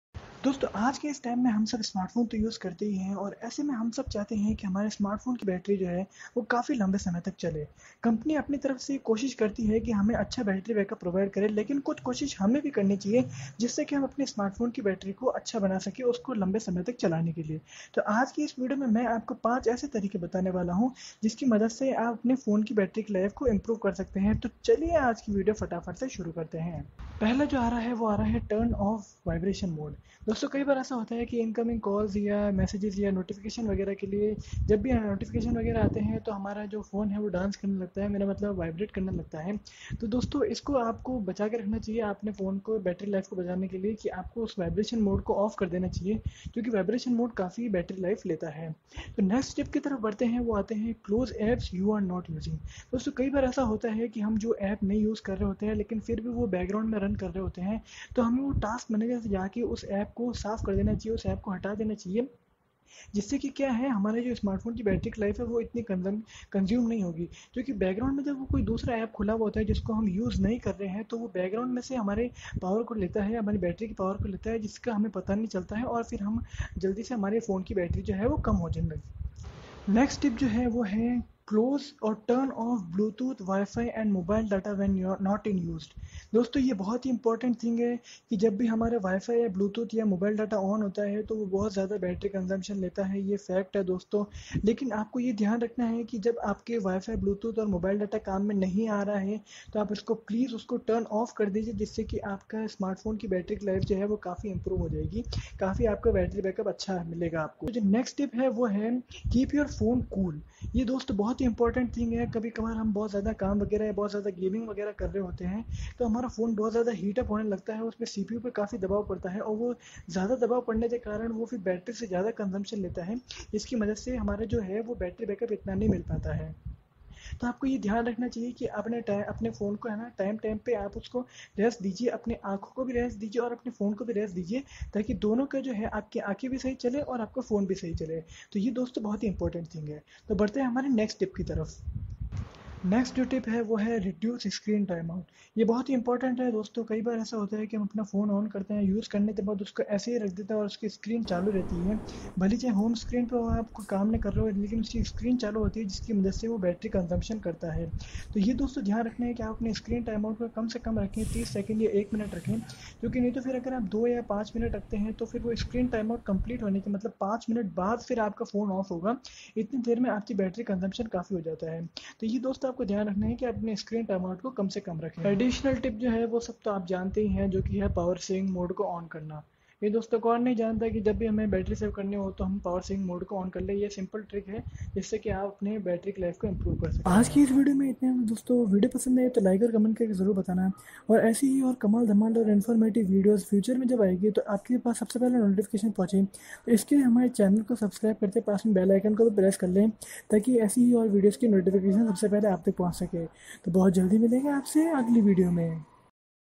5 Tips To Improve Your 🔋 Battery Life In HINDI 2020 | ये 3 मिनट आपके फ़ोन को पूरी तरह से बदल देंगे!
Hello dosto aap sabka swagat hai is video mein . To aaj ki is video mein mein aapko batane wala hoon ki aap apne phone ki battery life ko kese improve kar sakte hai in 5 asaan tareeko se

Video se related koi bhi doubt ho ya koi doosre doubt bhi ho to woh bhi comment kar dena hum zaroor reply karenge.